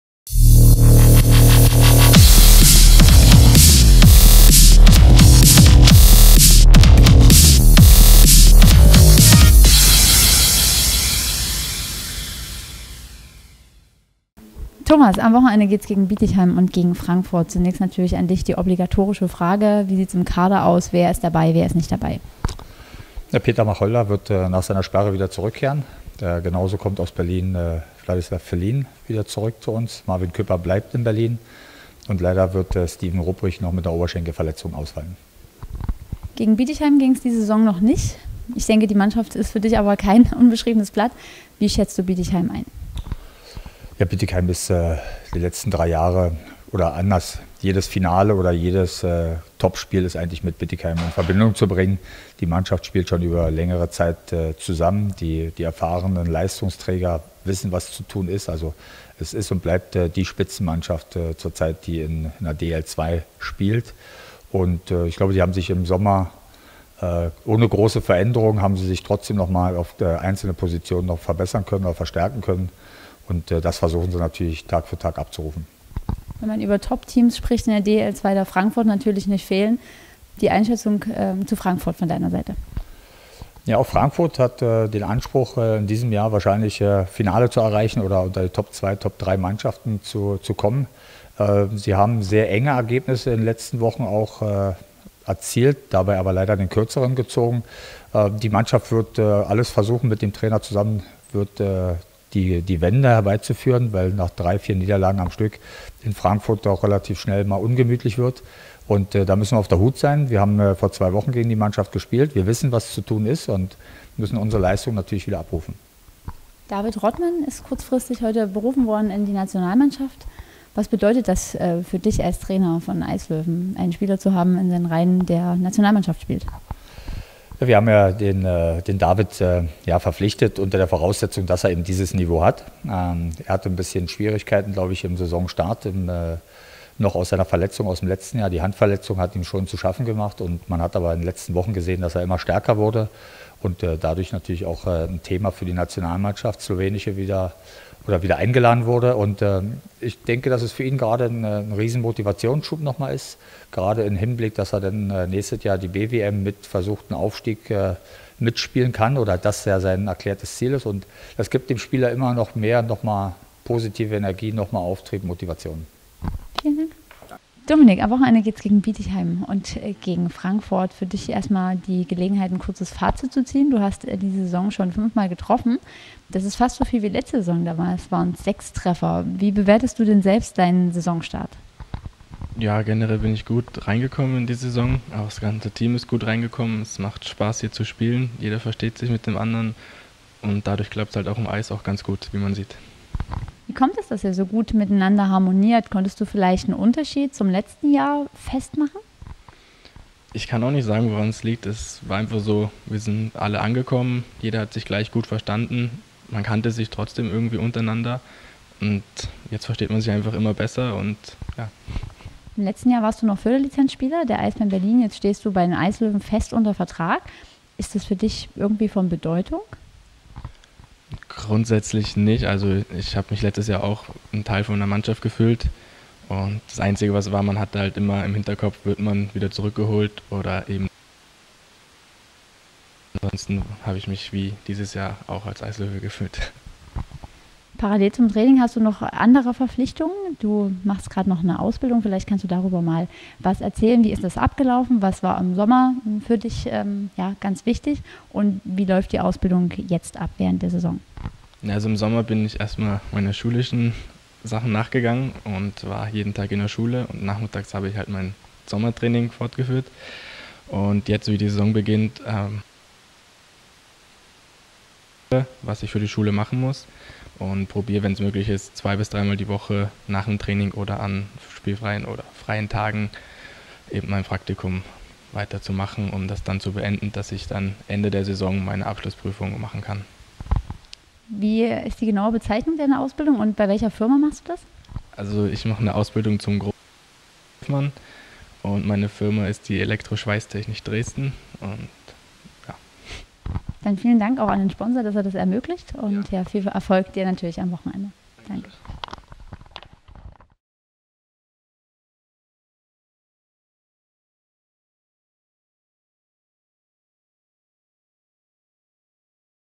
Vorschau auf den 13. und 14. Spieltag der DEL2 | Dresdner Eislöwen | 22.10.2015
Vorschau auf die Spiele

13. Spieltag:
Bietigheim Steelers vs. Dresdner Eislöwen

14. Spieltag:
Dresdner Eislöwen vs. Löwen Frankfurt

mit Thomas Popiesch und Dominik Grafenthin